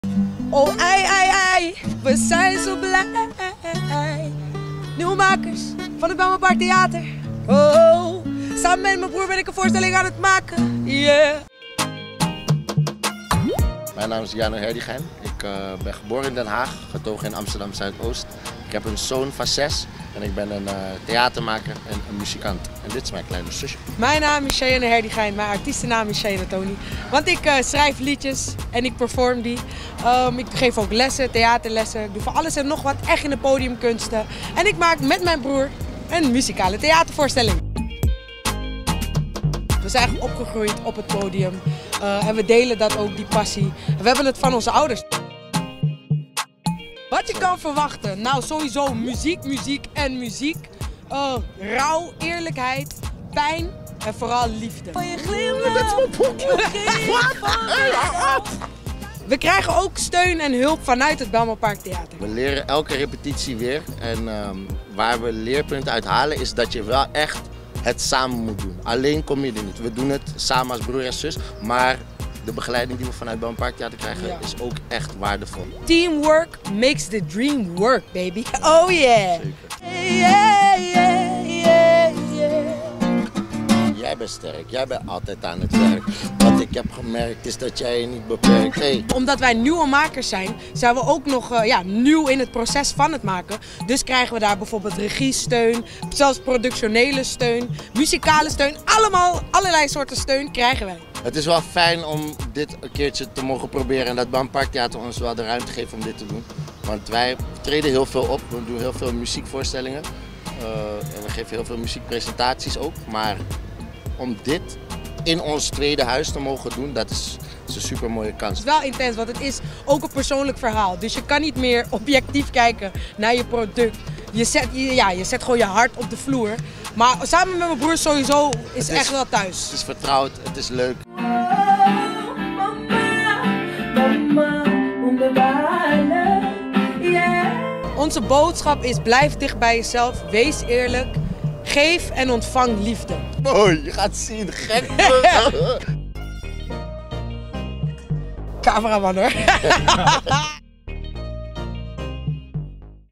Hoge Ogen: maak kennis met Cheyenne 'Toney' Herdigein en Guillano Herdigein [BIJLMER PARKTHEATER]
Hoge ogen is ons nieuwe makers-traject waarbij de nieuwste makers hun eigen voorstelling ontwikkelen, samen met een team van professionals uit verschillende vakgebieden binnen de theatersector. Tijdens dit weekend, nemen onze nieuwste makers het podium over om hun voorstelling.

Met trots stellen wij onze nieuwste makers voor: Cheyenne "Toney" Herdigein en Guillano Herdigein. Achter de schermen hebben deze nieuwste makers al veel geleerd en gerepeteerd, we kijken uit naar hun presentaties!